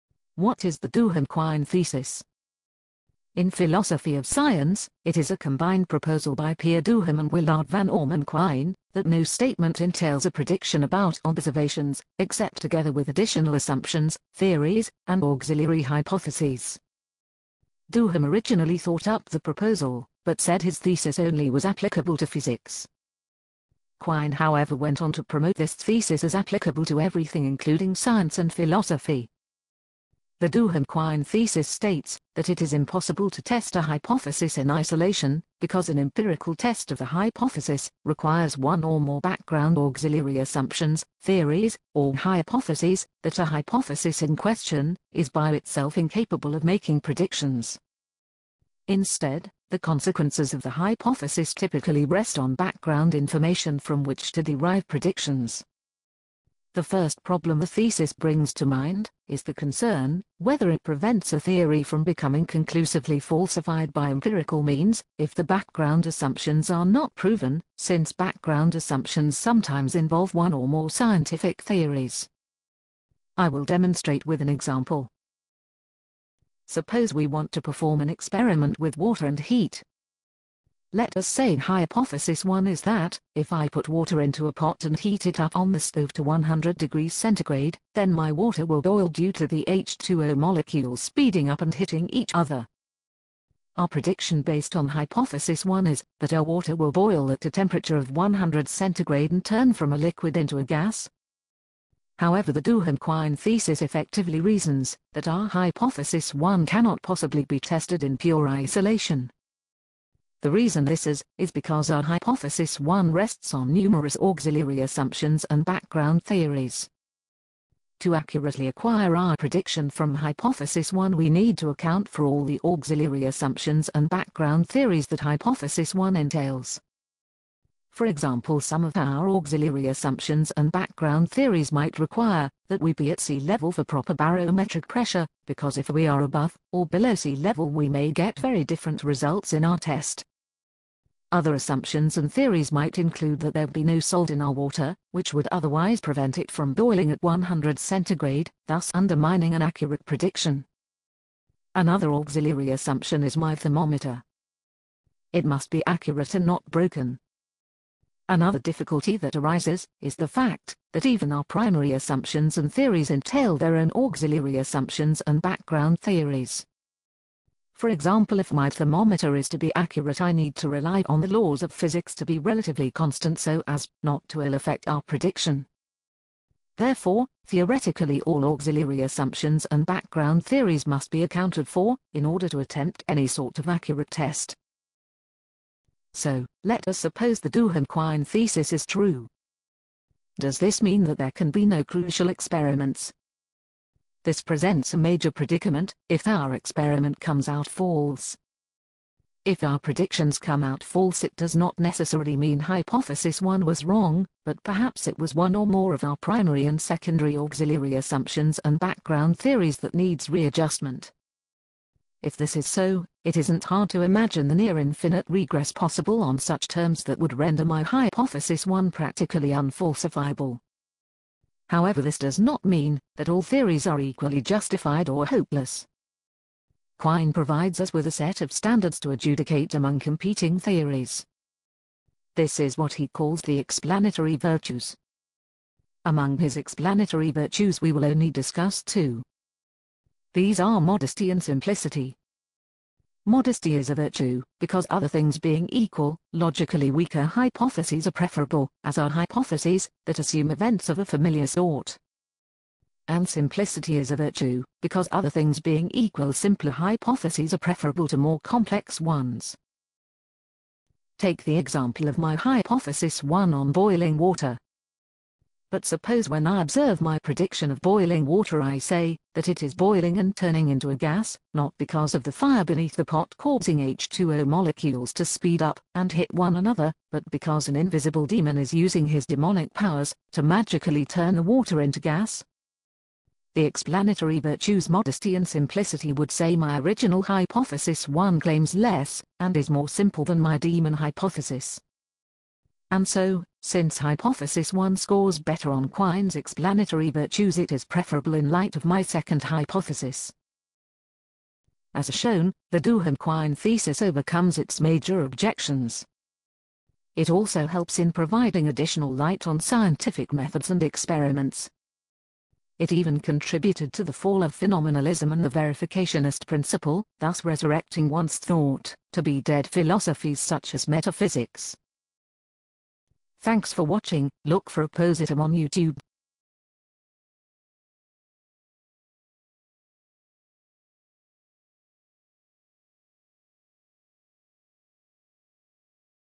Philosophy of Science: Duhem-Quine Thesis
I briefly explain in what the Duhem-Quine Thesis is, and argue that it overcomes a major objection, involving assistance from Quine's explanatory virtues.